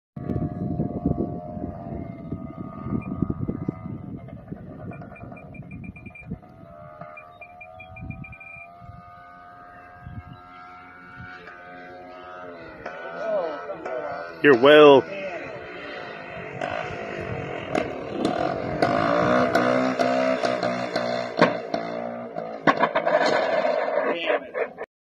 Crash landing of an RC Miss America P-51
15 approaches and go arounds trying to slow this beast down without any flaps. final attempt and everything was perfect. she was slowing down nice, tracking straight, RPM's coming down finally and pop there goes the wheel aborting the mission while the P51 has no other choice but to lay down.